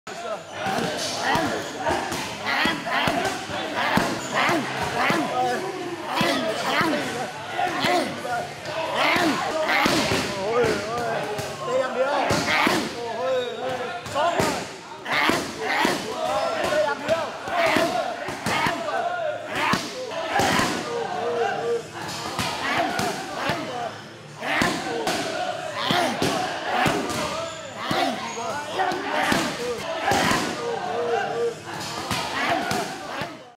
Muay Thai World Champion Dejdamrong Sor Amnuaysirichoke Puts In Work!
Multiple-time Muay Thai World Champion Dejdamrong Sor Amnuaysirichoke from the EVOLVE Fight Team puts in work with fellow multiple-time Muay Thai World Champion Penek Sitnumnoi at the Evolve Fighters Program!